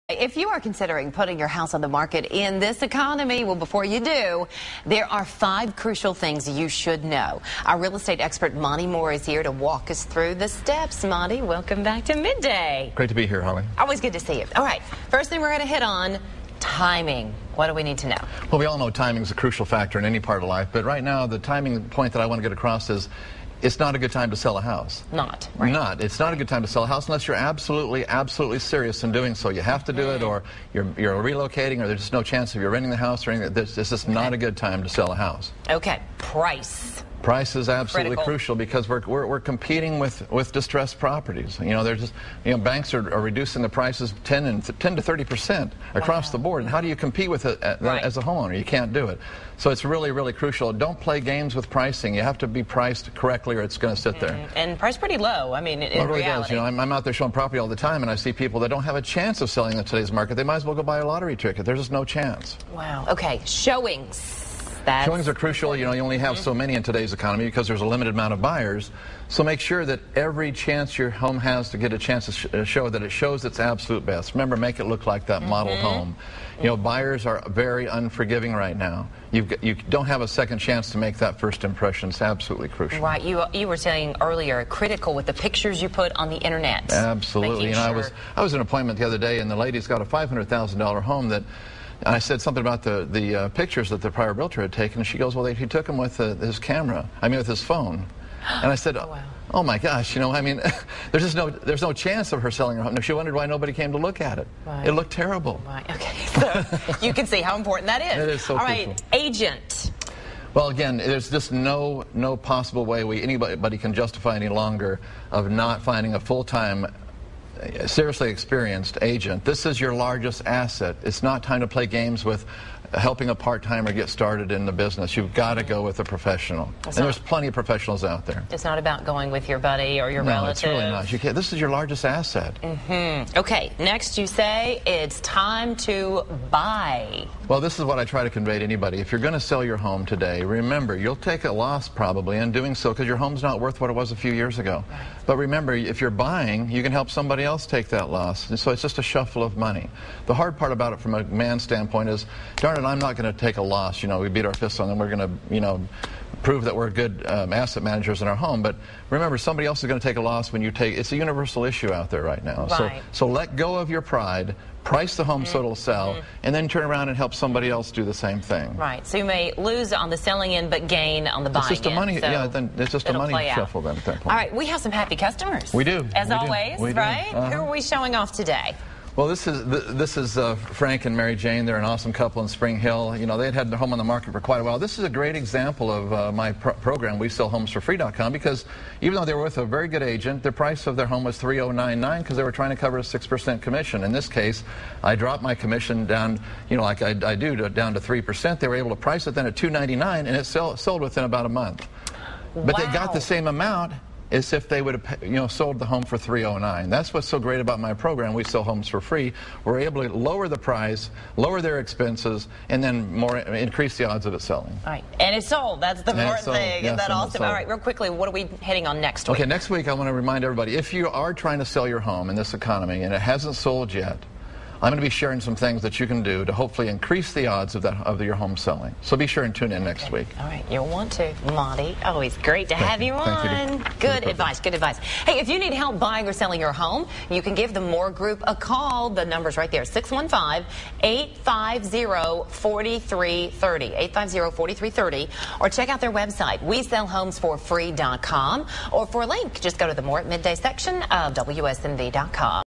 Top Spring Hill TN Real Estate Agent: 5 Things Before Putting Home on Market
Call Monte at (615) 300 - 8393. Top Spring Hill TN Real Estate Agents Talks on 5 Things You Should know Before Putting Your Home on Market.  If you are looking for a great realtor in Spring Hill, Tennessee then you want to take your time and interview the right people.

If you have any real estate questions, call or stop by!

EXIT Realty The Mohr Group & Associates
600A Frazier Drive, Suite 123